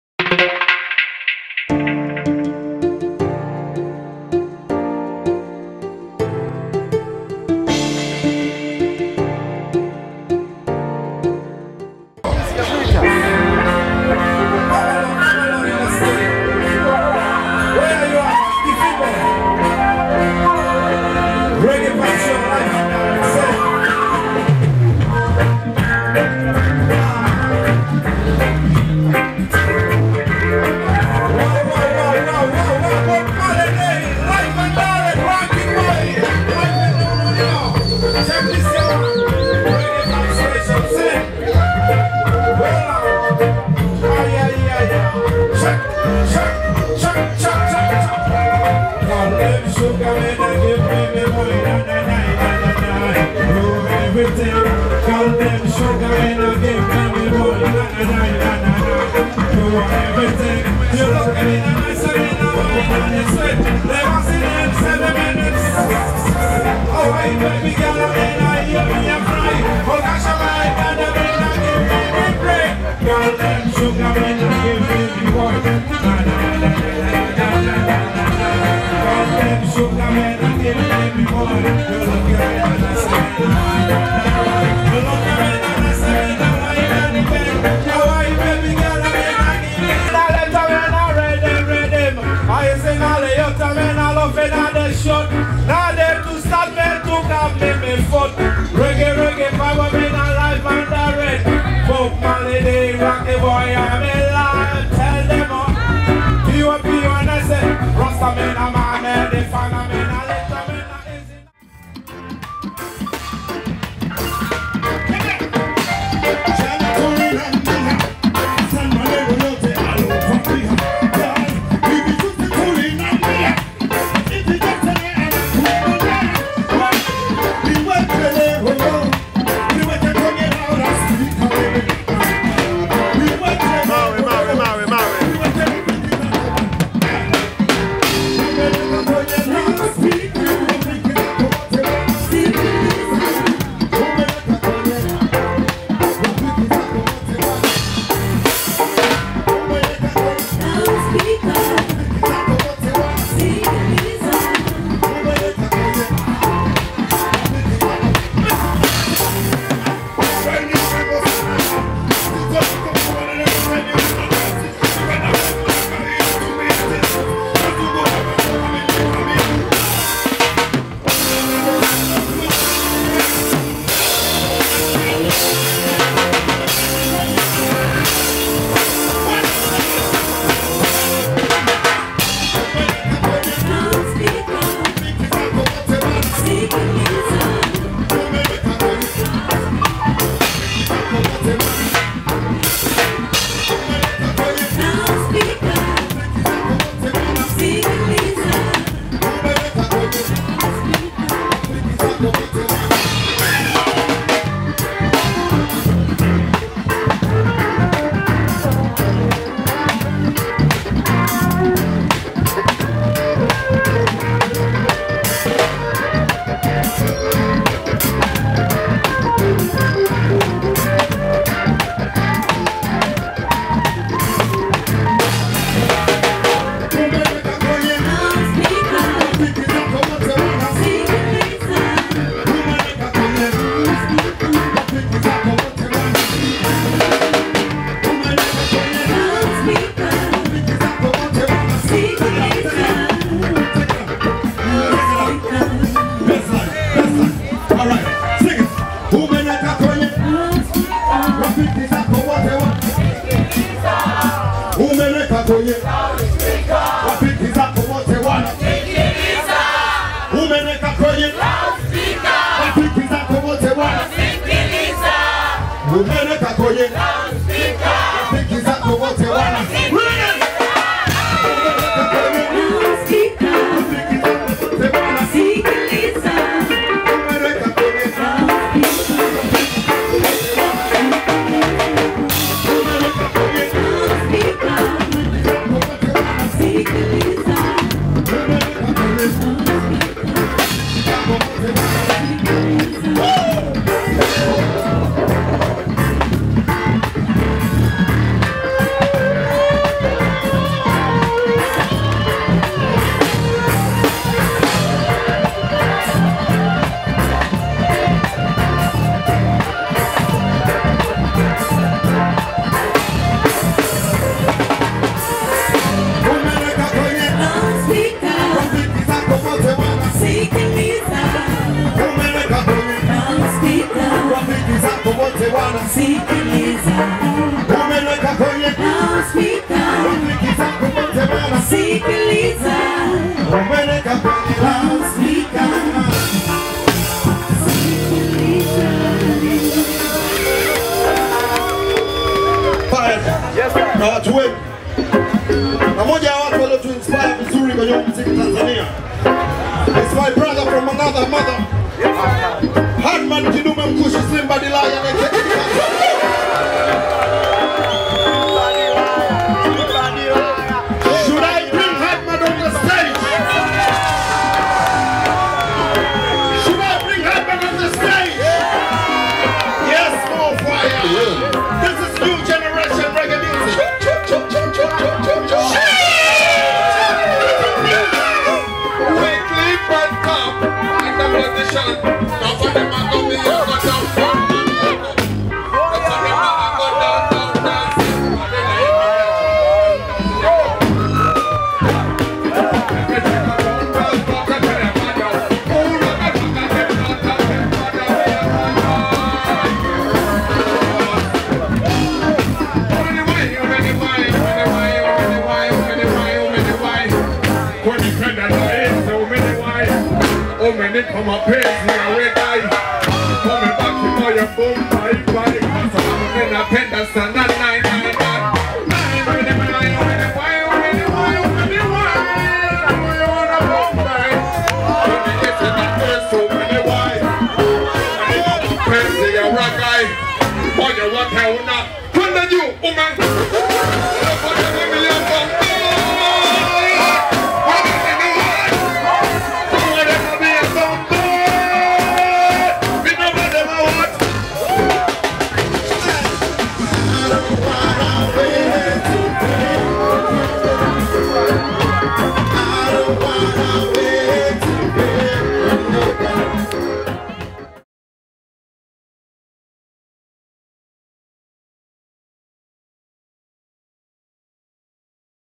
BOB MARLEY DAY: Dabo Mtanzania performing @ Reggae Bar - Ununio. (May 2021)
Robert Nesta Marley,  born 6 February 1945 and passed on 11 May 1981; was a Jamaican singer, songwriter, and musician who is considered one of the pioneers of reggae music.
Marley's contributions to music is worldwide, and made him a global figure in popular culture for many decades.

Enjoy the videos and music you love, and share it all with friends, family, and the world on social media YouTube.